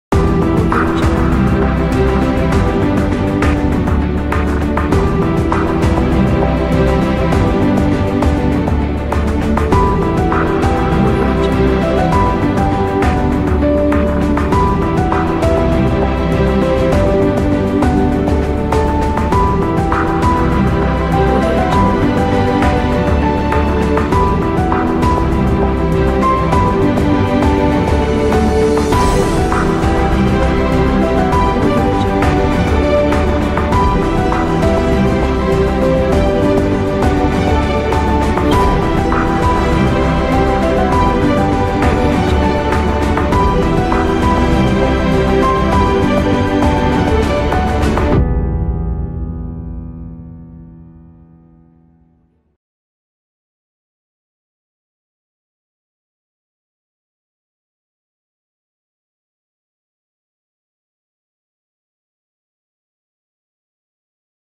Cinematic Media Opener (AfteR Effects template)
Cinematic Media Opener is a high quality, well organized and easy customizing template. This project has modular structure, so you can easily change duration. Project includes video tutorial, even if you first time run program after effects you can easily complete job. Just drop your image or video, edit the text, add audio and enjoy result!